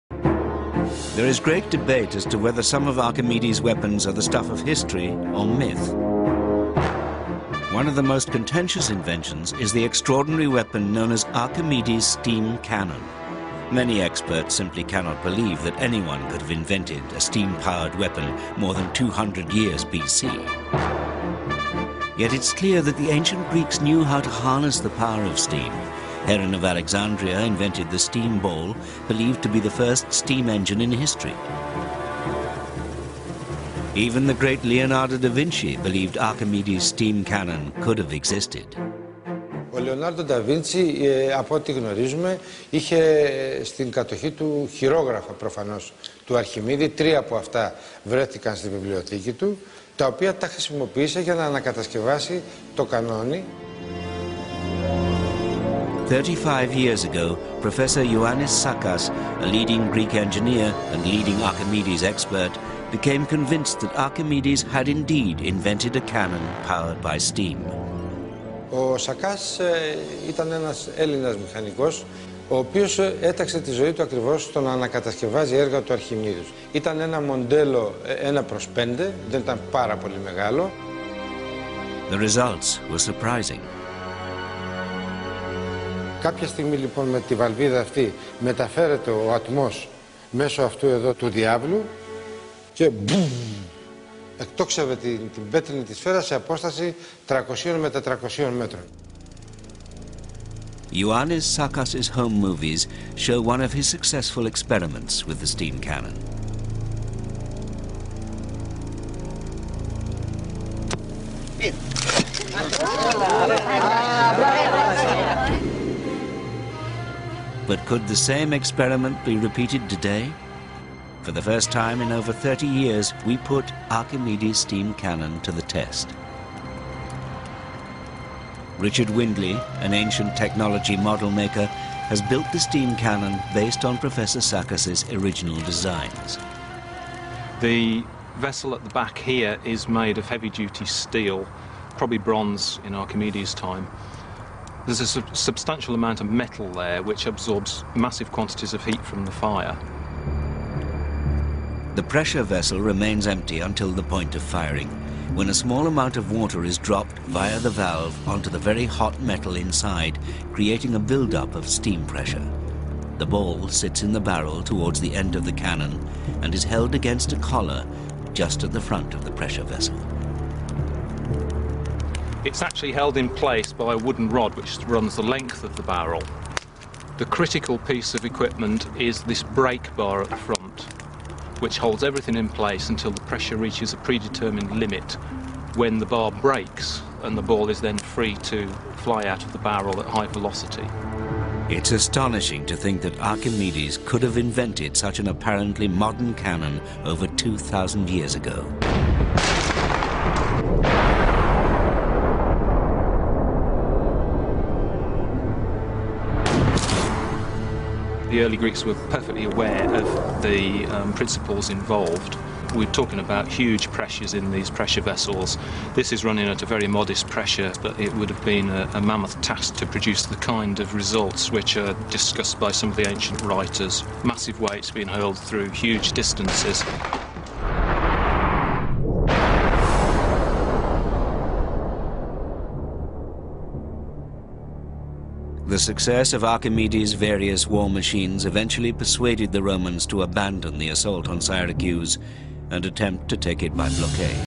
Archimedes steam cannon
Leonardo Da Vinci: Architronito e una macchina di fino rame, invenzlon d' Archimede.

The Cicero manuscript later was used by Leonardo Da Vinci who called the device "Architronito" in honour of Archimedes. He produced only drawings of the steam gun but Ioannis Sakas, a Greek expert of the work of Archimedes used this information to build a test device in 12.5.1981. A vessel was heated and when it reached 400 degrees Celsius 6 g of water was enough to produce in 10 seconds steam that expanding could throw a tennis ball size stone 50 meters. The reconstructed Archimedes steam gun by Sakas was only 1/5 the size of the original.